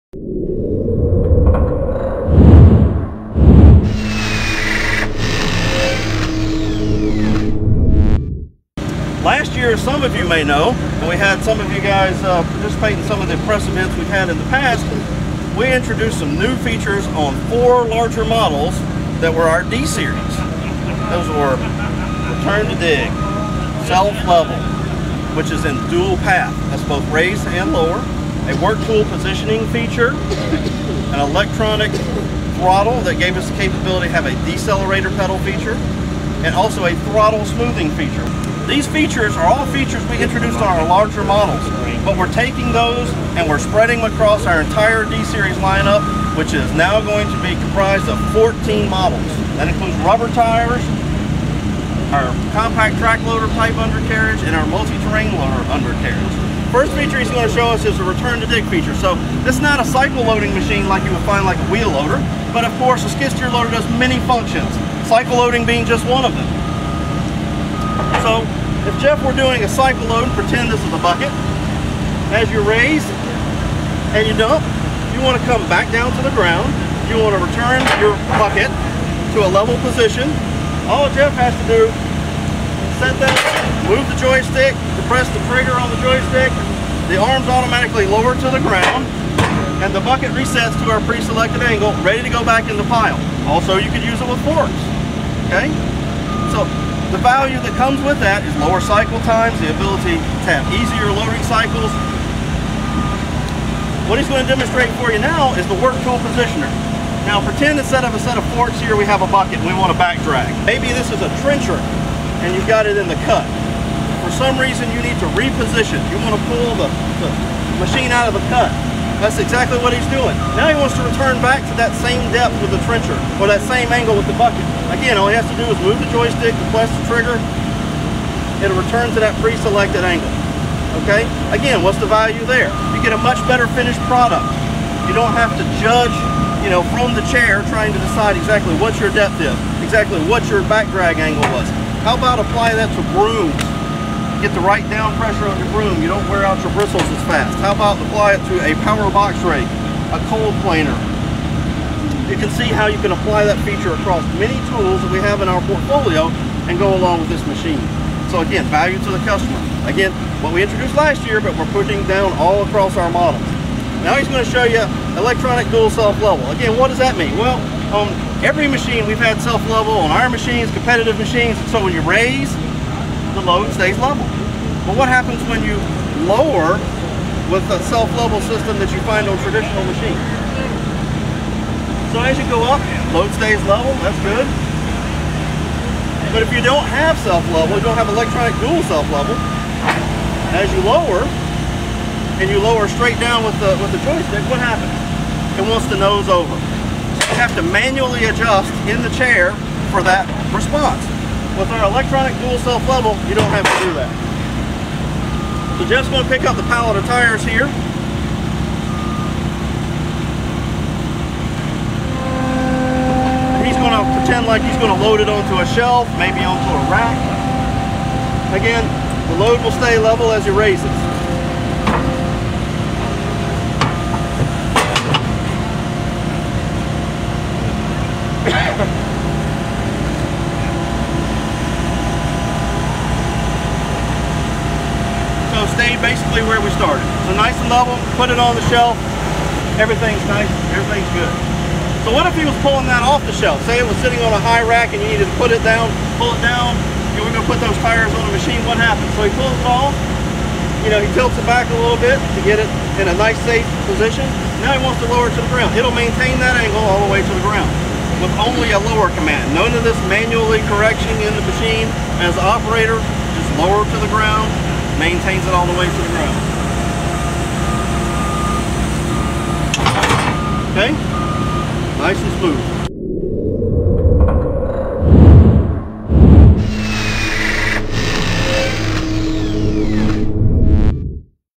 New Cat D Series Skid Steer Auto Loader Functions
FCP video (4:41 min.) shows Caterpillar demonstrating electronic return to dig, work-tool positioner, and dual self-leveler features built into the new D Series skid steers and compact track loaders